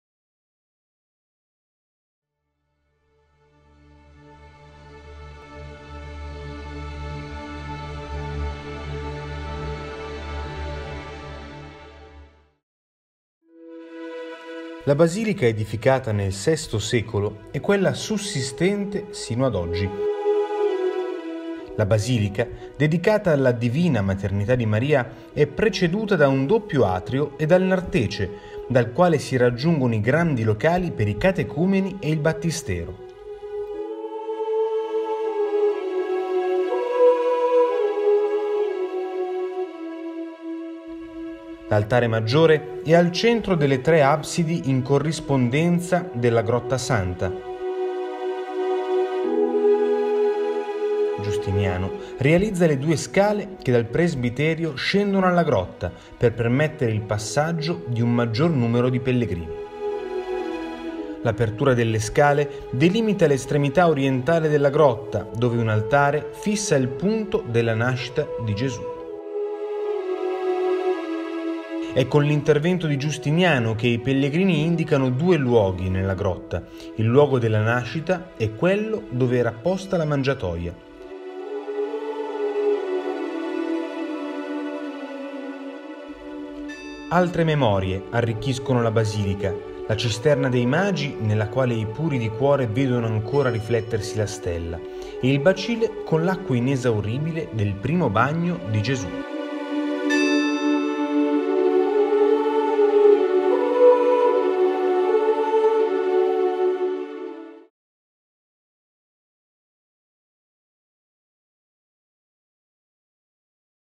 La Basilica della Natività nel VI secolo - Betlemme back in time
Terza puntata di Betlemme back in time! Il nostro viaggio ci porta nel VI secolo, nella Basilica giustinianea.
Insieme ai pellegrini del tempo scopriremo la ricchezza e il fascino della tradizione. Troveremo ogni in ogni angolo della basilica il ricordo di un evento importante, come la cisterna dei Magi dove i puri di cuore potevano ancora vedere riflessa la stella e molti altri ancora!